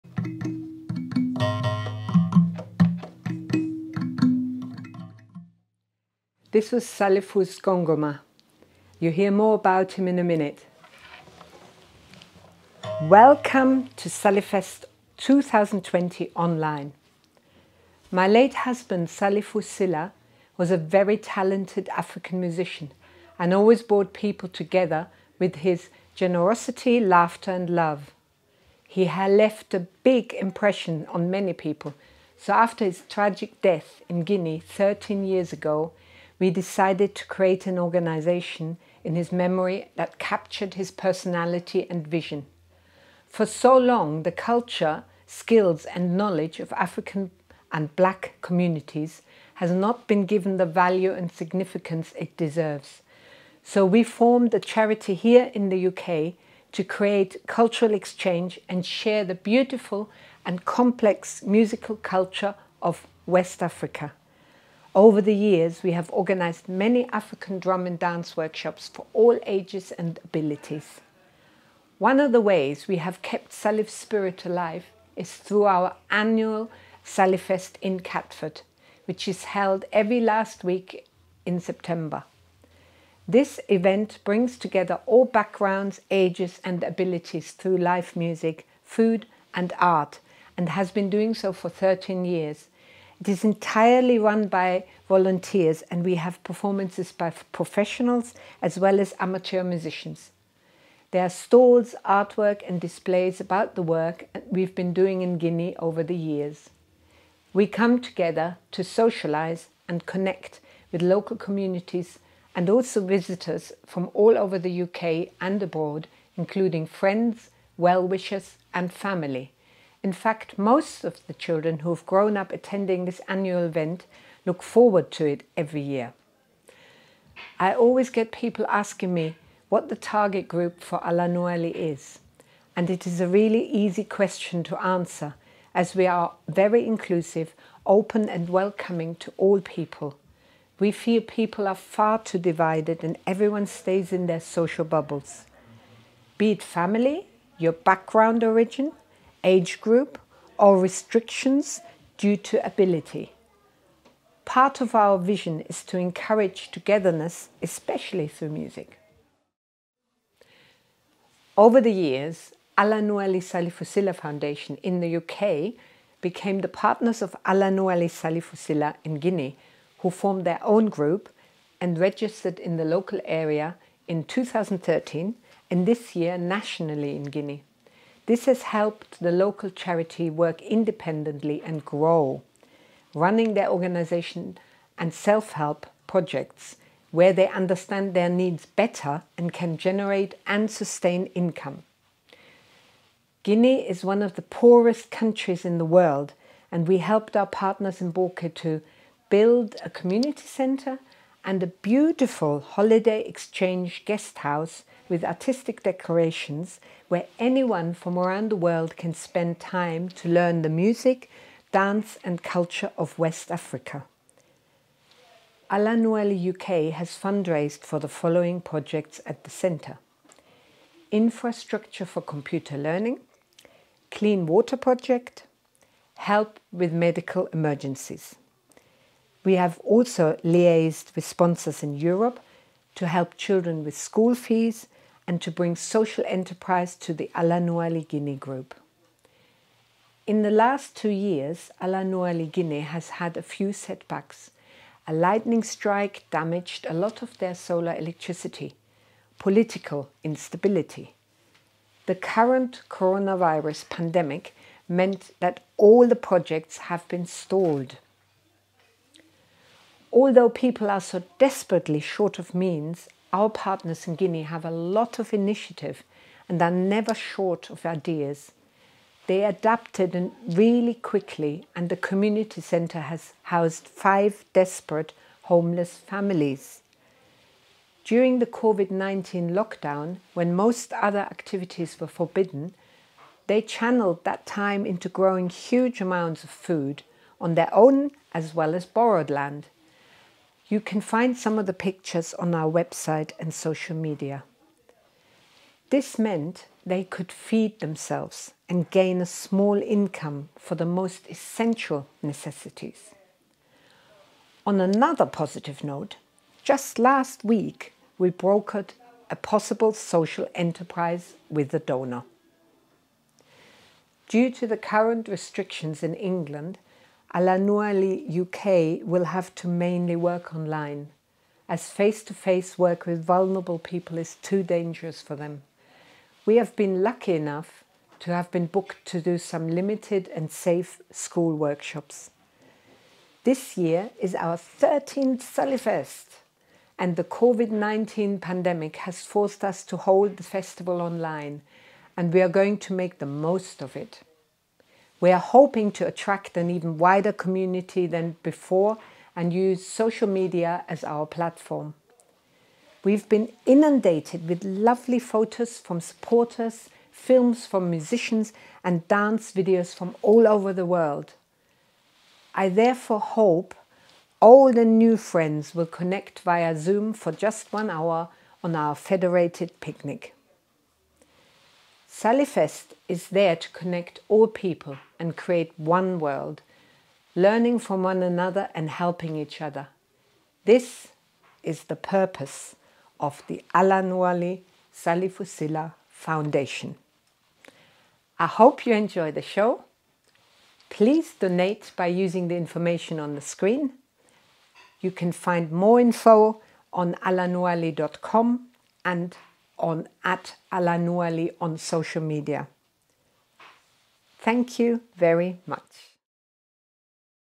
Hanna explains how Alanouwaly is doing in a film for Salifest 2020
What the Alanouwaly Salifou Sylla Foundation is about laid out by Hanna, the operational director. She is talking about the successes and challenges and where Alanouwaly is heading.